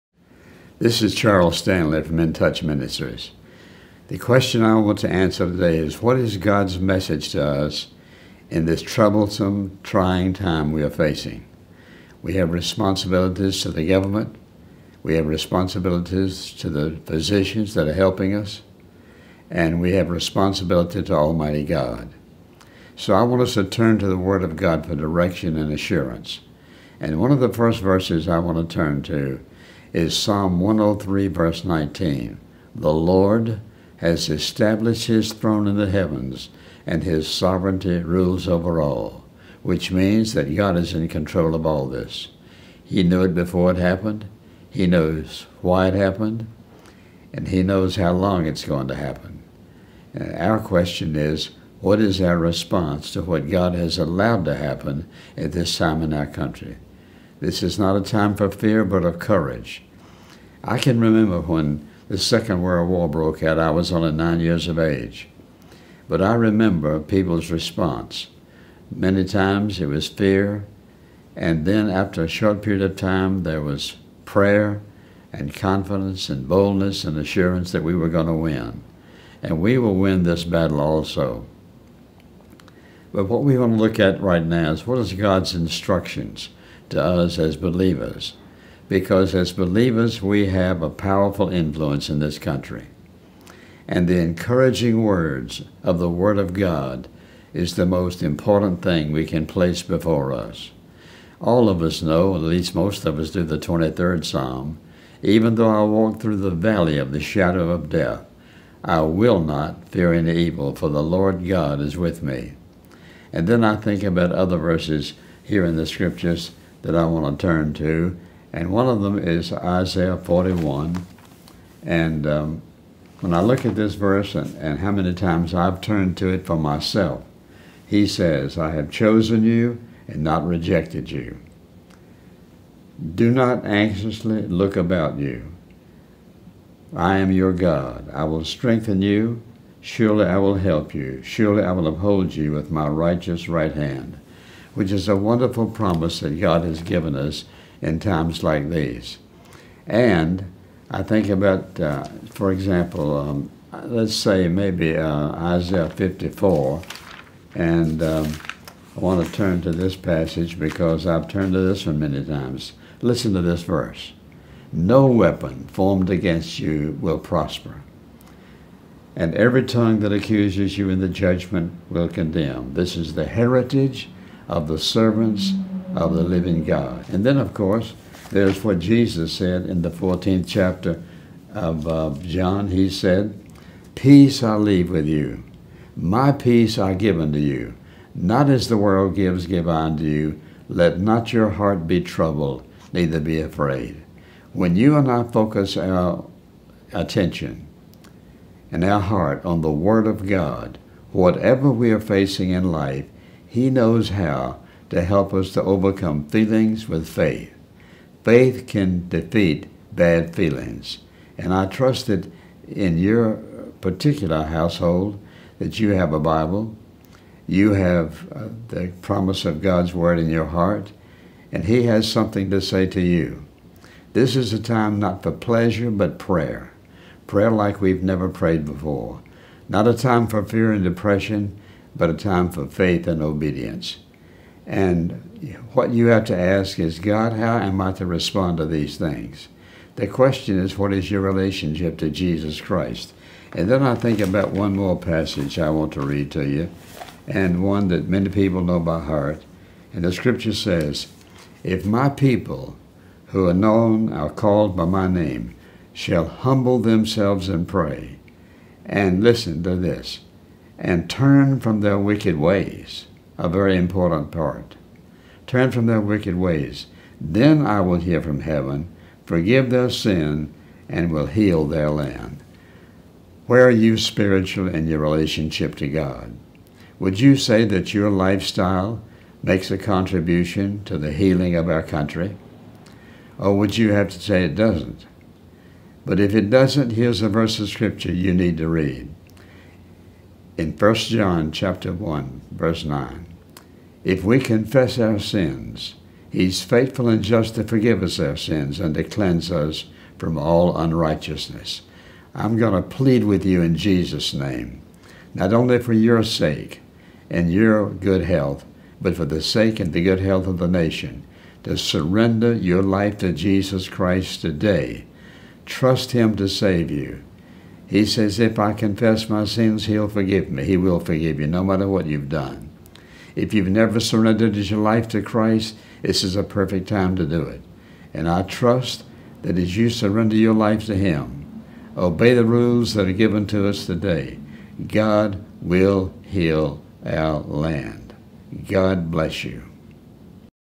Faith Over Feelings COVID 19
Don't allow the Corona Virus (COVID 19) to  hurt you mentally Message from Dr  Charles Stanley